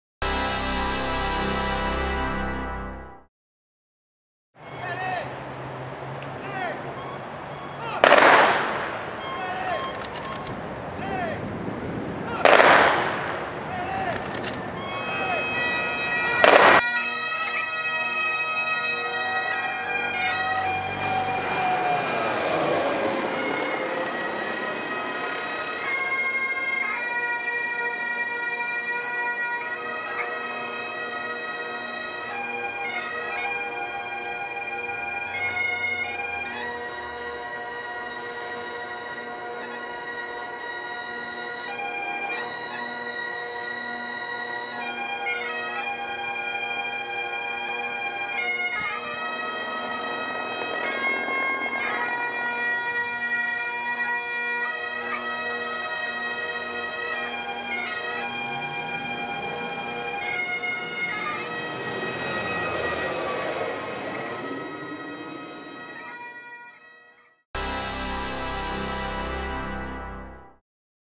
Patriot Day Fly Over 9 11 2017
A skilled HB1 pilot, Mark Wersching  caps off this year's Patriot Day event at Pier Plaza, complete with rifle salute. See the Surf City Highlight later including Officers of the Year, and more bagpipes on HBTV3 and on the City's YOUTUBE channel! Thank you to the amazing American Legion Post 133!